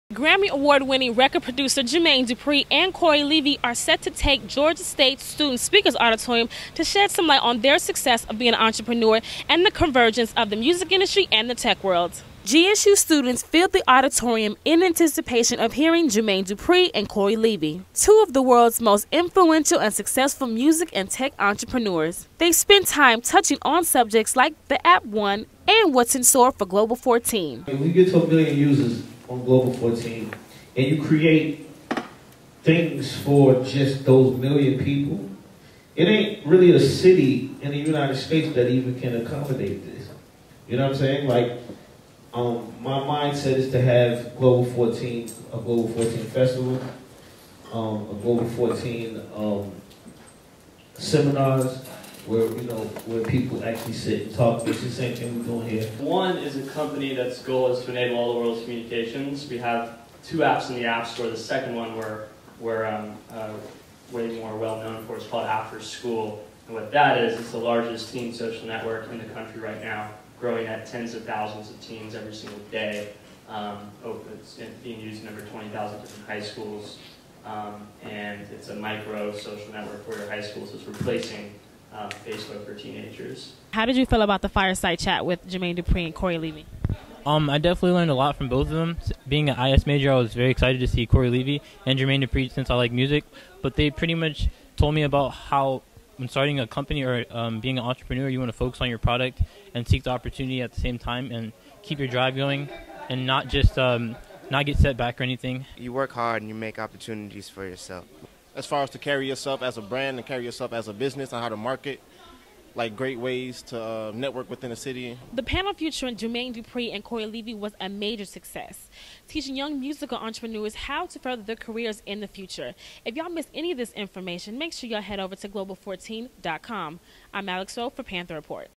Jermaine Dupri and Cory Levy on the music industry
Jermaine Dupri and Cory Levy spoke with students in the Speakers Auditorium about what they can do to better peruse their career in music.